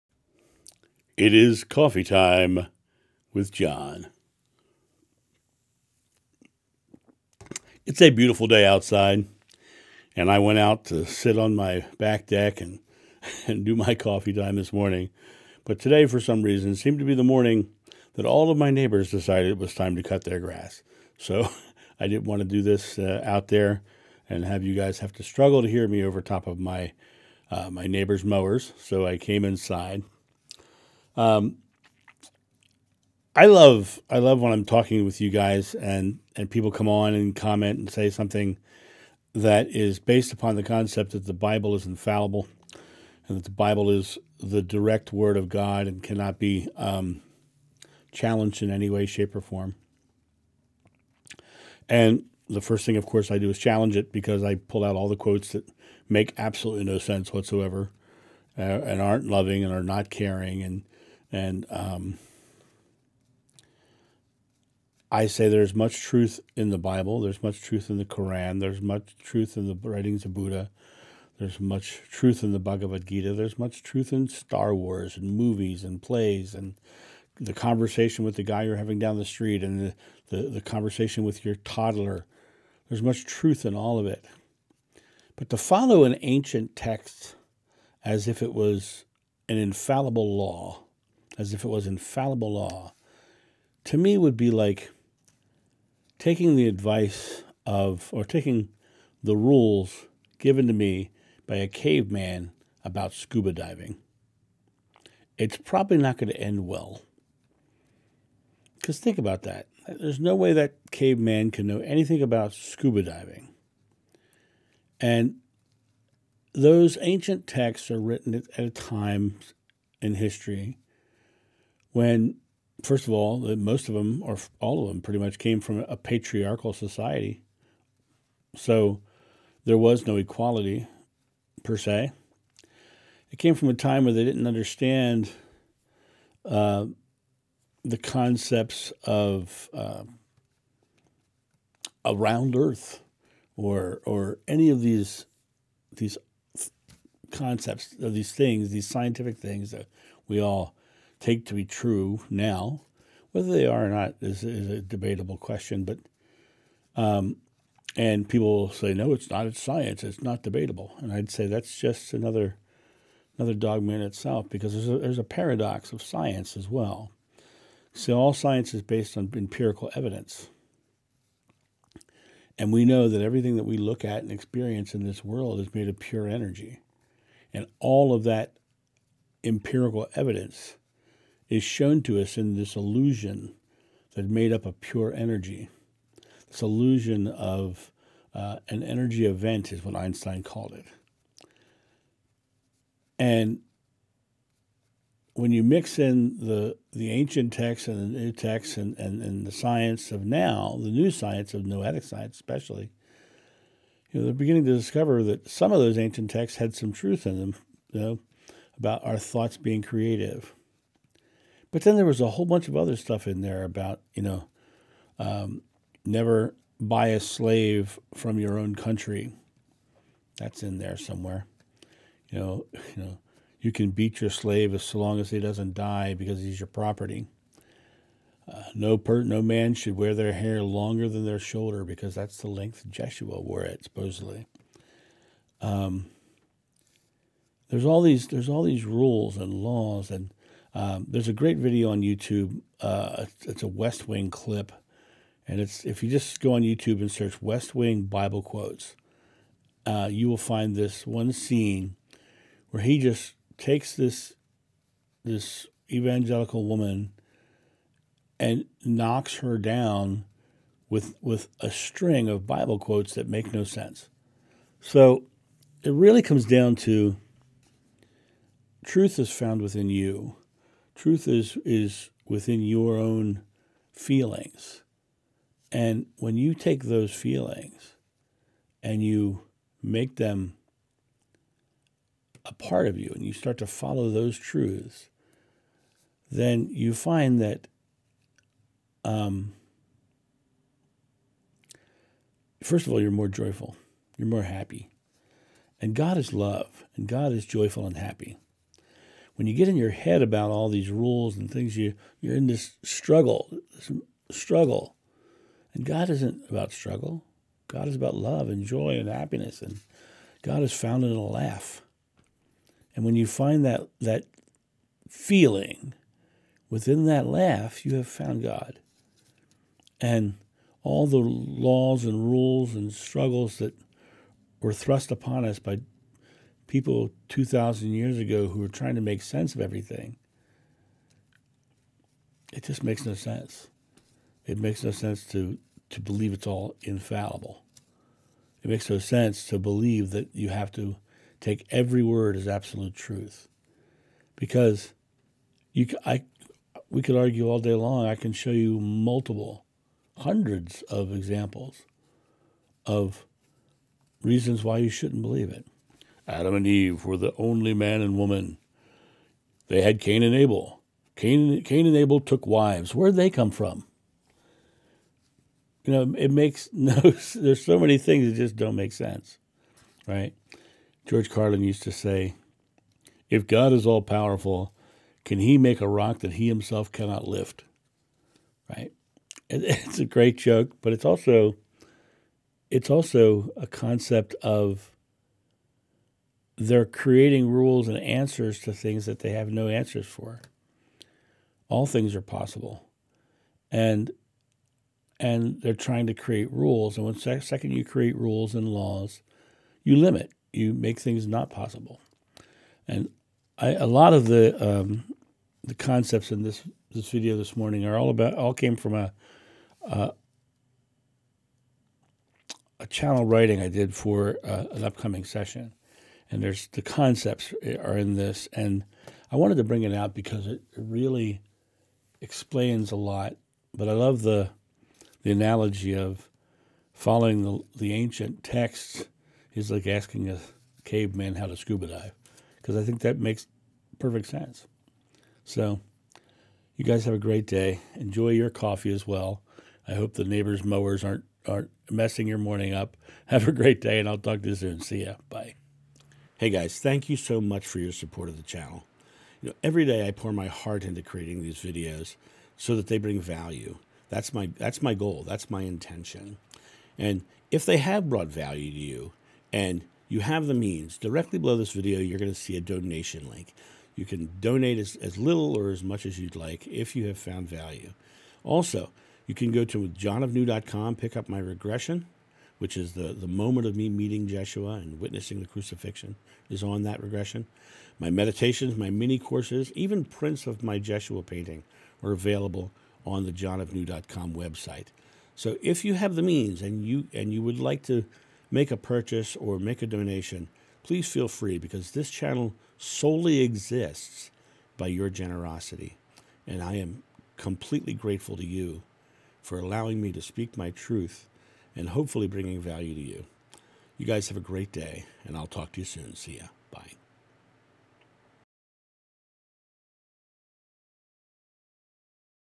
Don't ask a caveman how to scuba dive
John discusses the futility of following ancient texts.